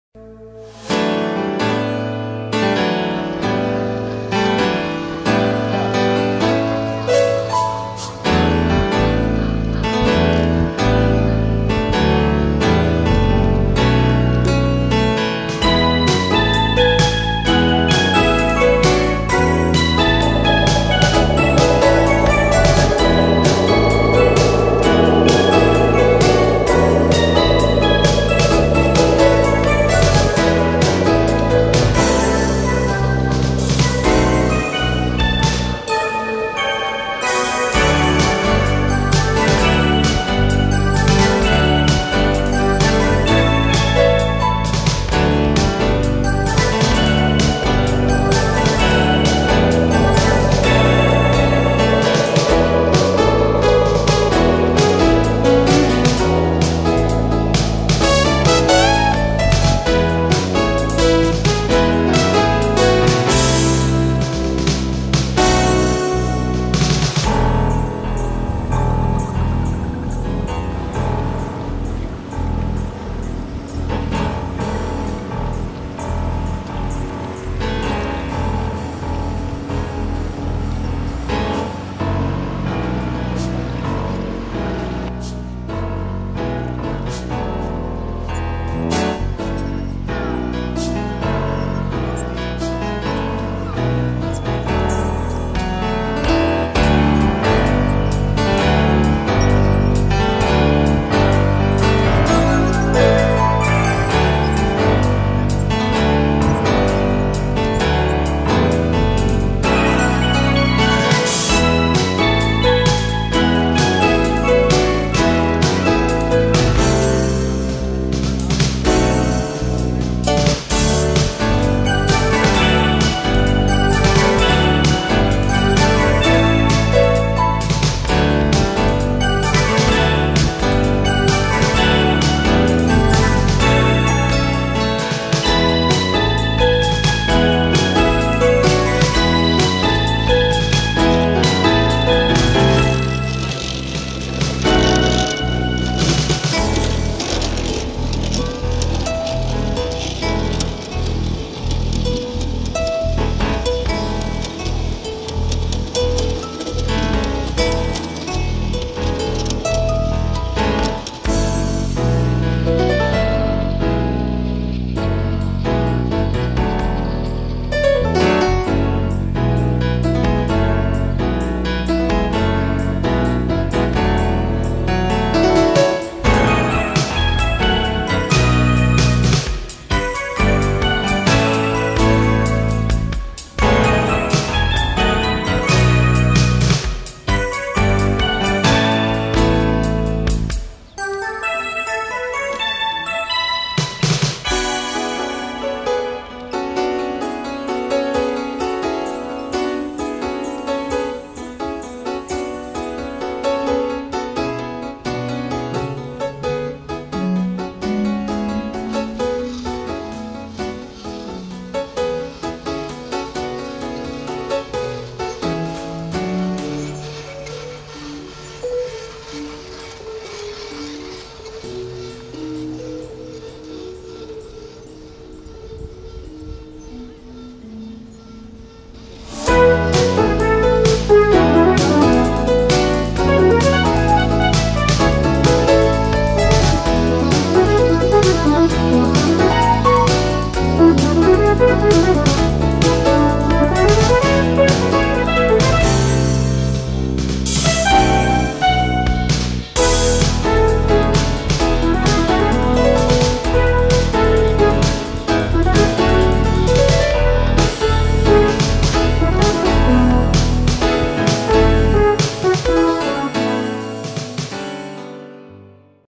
Minimundus Klagenfurt: Die Eisenbahnen
Die Eisenbahnen der Miniaturwelt "Minimundus" in Klagenfurt am Wörthersee.